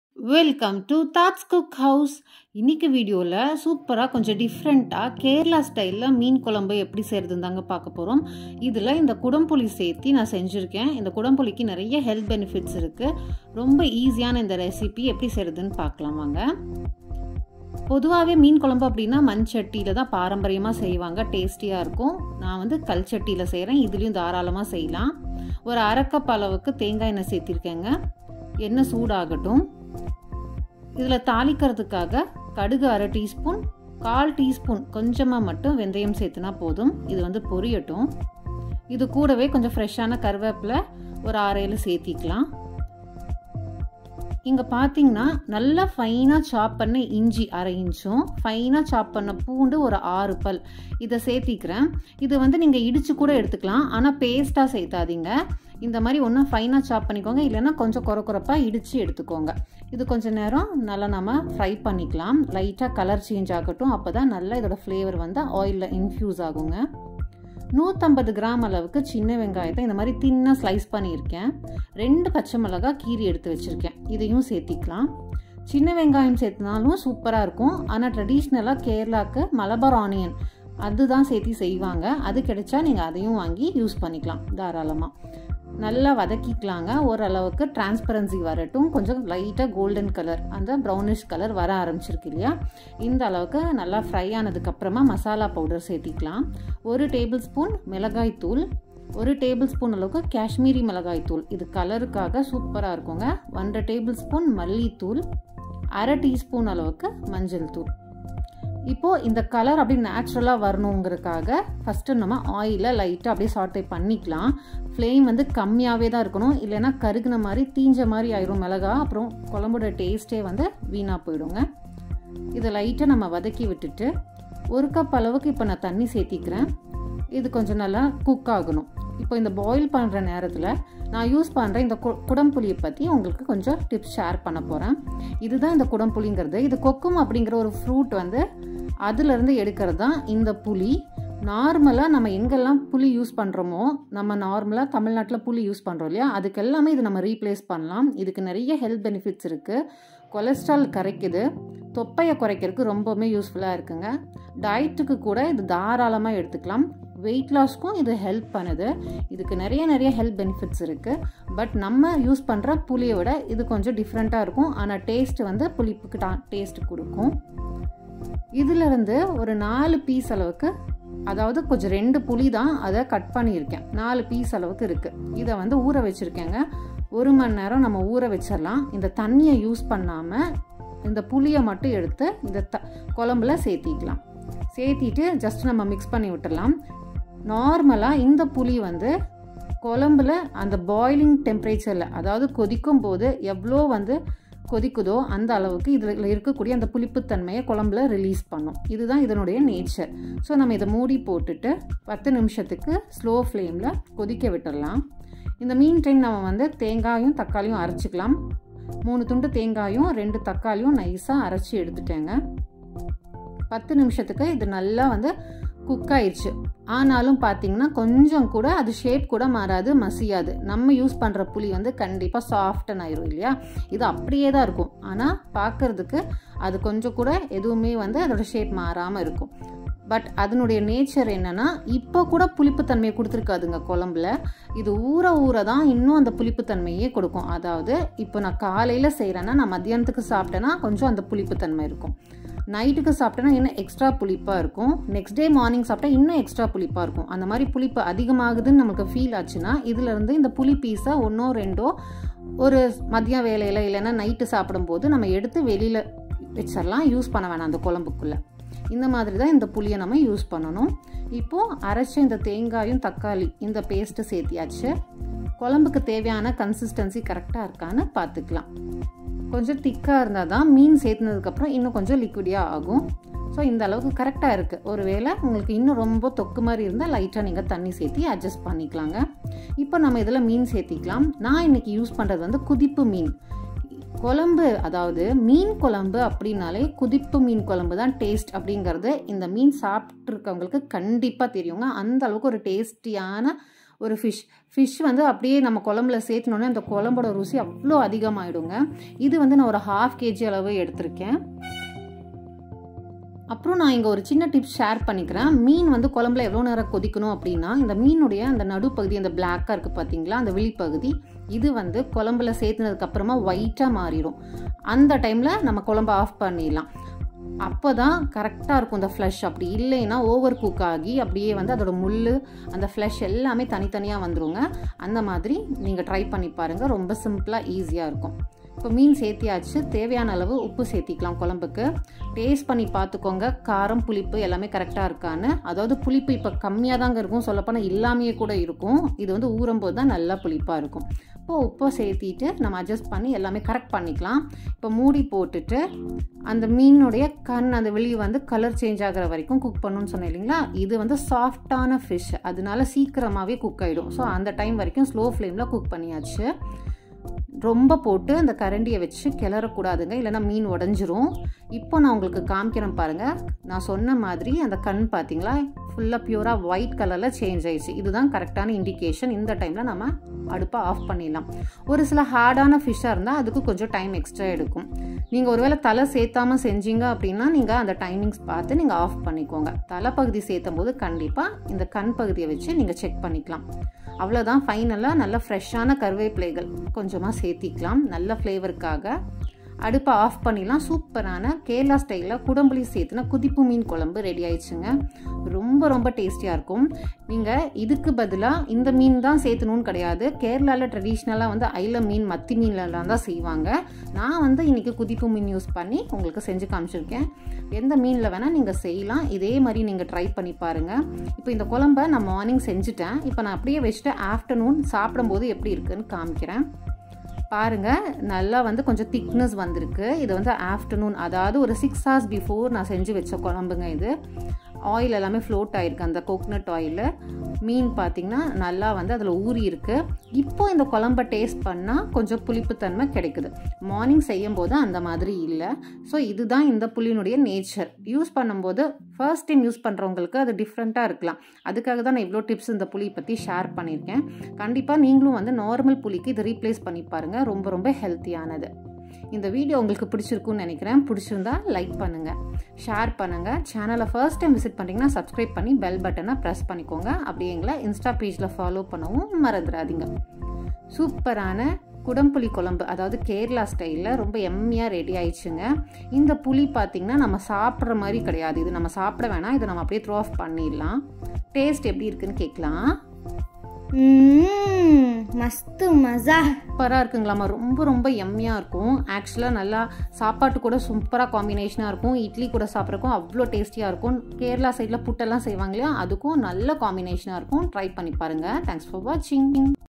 Kerala style kudampuli fish curry recipe in tamil | Kuthippu meen Kuzhambu | Taj's Cookhouse
Hello everyone 😊
In this video I have shown how to make traditional Kerala style fish curry. In this recipe i have used Malabar tamarind             ( kudampuli in tamil ) and shared the health benefits of this ingredient. Hope this video is helpful for everyone.

Your subscription means a lot to me 🥰
Happy dining!!!

Ingredients:
1/2 kg fish
1/2 cup coconut oil
1/2 tsp mustard seeds
1/4 tsp Fenugreek seeds
Some fresh curry leaves
1/2 inch ginger
6 garlic cloves
150 g shallots
2 green chillies
1 tbsp chilli powder
1 tbsp kashmiri chilli powder
1.5 tbsp coriander powder
1/2 tsp turmeric powder
4 pieces Malabar tamarind
3 pieces coconut
2 tomatoes
1/2 kg fish
Required salt
Required water

Chapters:
0:00-Intro
0:16-Making kerala style fish curry
2:15-Health benefits of kudampuli
4:42-Tasty kuthippu meen Kuzhambu
8:00-Outro